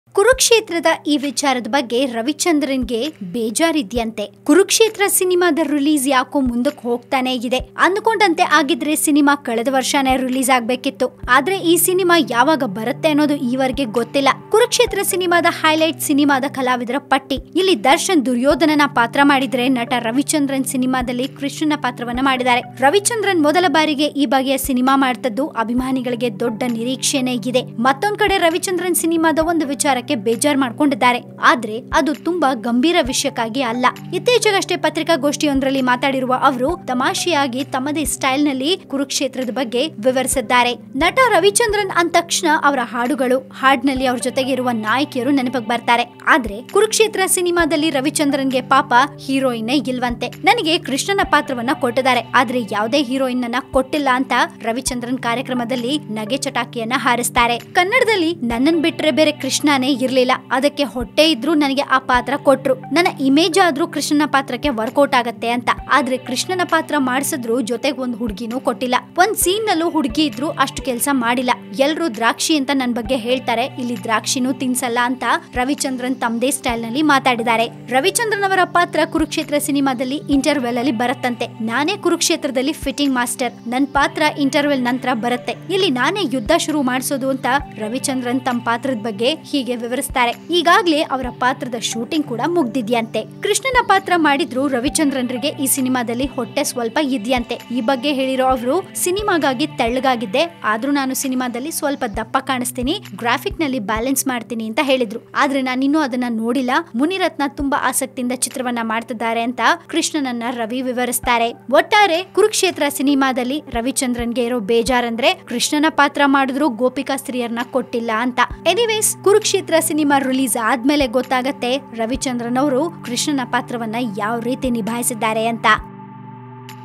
Kurukshetra Movie : ಕುರುಕ್ಷೇತ್ರ ಸಿನಿಮಾದ ವಿಚಾರಕ್ಕೆ ರವಿಚಂದ್ರನ್ ಗೆ ಬೇಸರ ಯಾಕೆ?
'ಕುರುಕ್ಷೇತ್ರ' ಸಿನಿಮಾದ ಬಿಡುಗಡೆ ಮುಂದಕ್ಕೆ ಹೋಗುತ್ತಲೇ ಇದೆ. ಅಂದುಕೊಂಡಂತೆ ಆಗಿದ್ದರೆ ಸಿನಿಮಾ ಕಳೆದ ವರ್ಷ ಬಿಡುಗಡೆಯಾಗಬೇಕಿತ್ತು. ಆದರೆ, ಈ ಸಿನಿಮಾ ಯಾವಾಗ ಬರುತ್ತೆ ಎನ್ನುವುದು ಈ ವರೆಗೆ ತಿಳಿದಿಲ್ಲ. ರವಿಚಂದ್ರನ್ ಸಿನಿಮಾದ ಒಂದು ವಿಚಾರಕ್ಕೆ ಬೇಸರವಾಗಿದ್ದಾರೆ.

Actor Ravichandran spoke about 'Kurukshetra' movie & about his Krishna role.